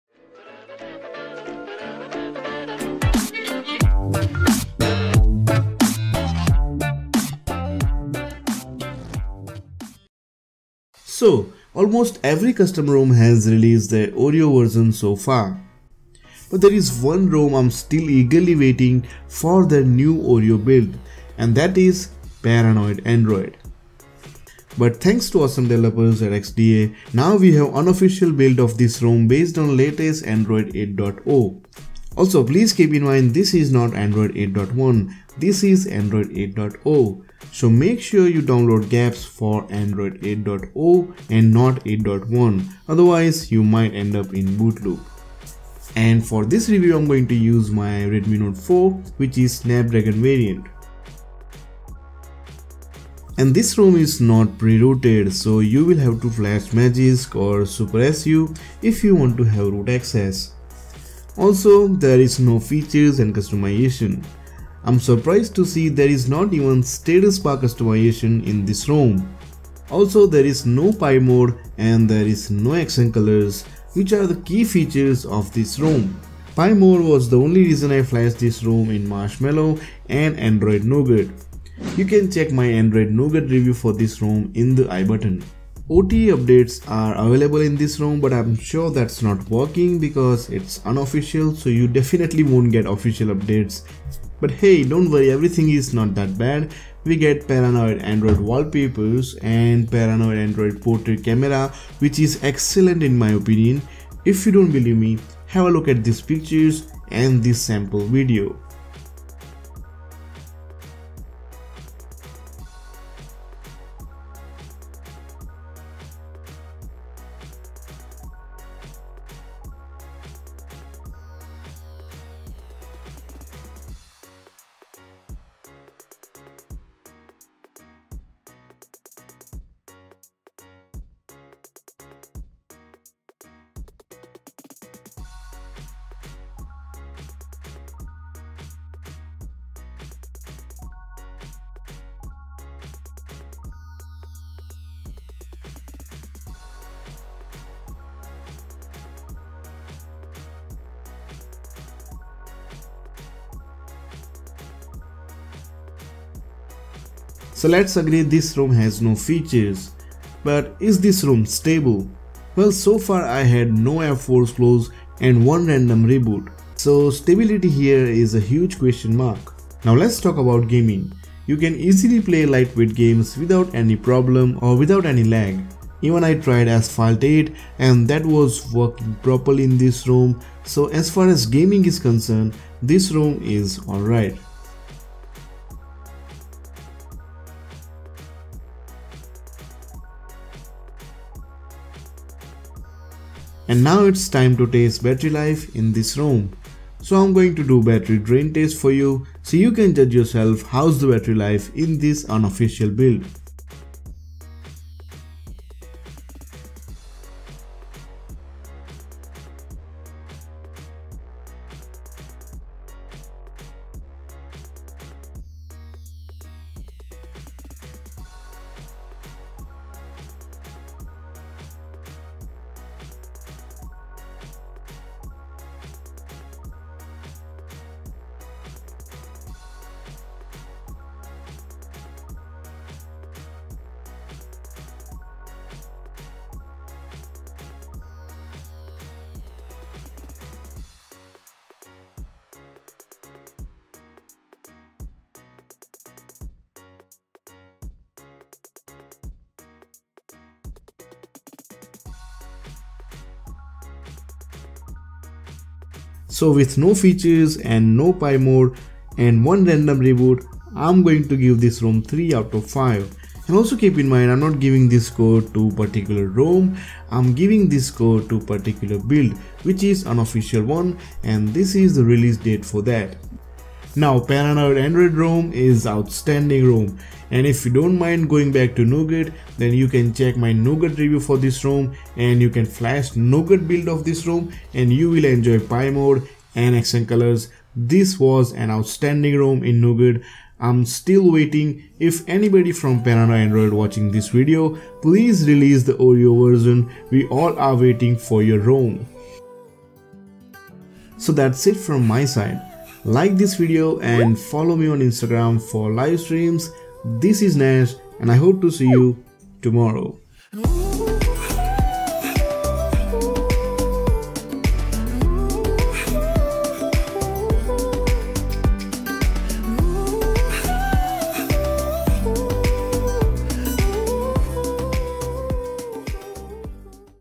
Paranoid Android Oreo 8.0 Rom Review 2018
paranoid android also known as aospa 8.0 oreo available for redmi note 4 its unofficial or official check out the video paranoid android 8.1 official oreo will release soon paranoid android bettter than lineage os or dot os ? Paranoid android oreo 8.0 rom full in depth review by game of roms

Want to Ask Me Something Lets Be Friends